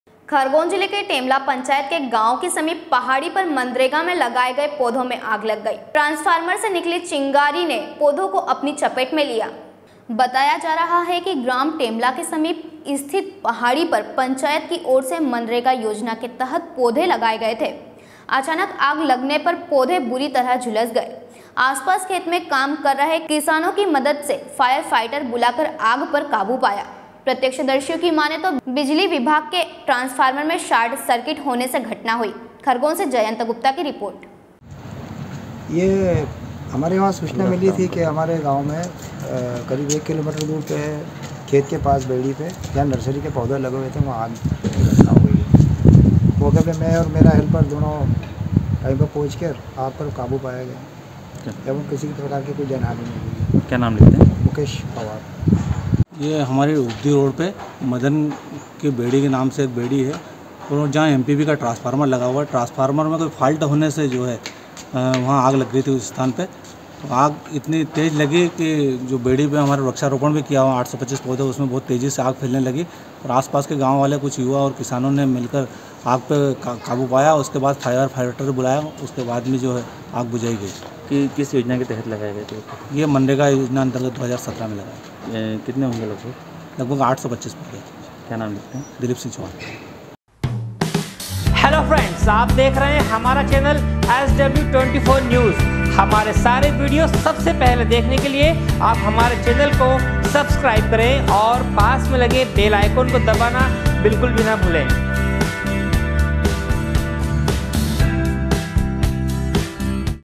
SW 24 News Madhyapradesh
खरगोन जिले के टेमला पंचायत मंे गांव के समीप पहाड़ी पर मनरेगा में लगाये गये पौधों मे आग लग गई। ट्रांसफार्मर से निकली चिंगारी ने पौधों को अपनी चपेट मे लिया। खरगोन से जयंत गुप्ता की रिपोट |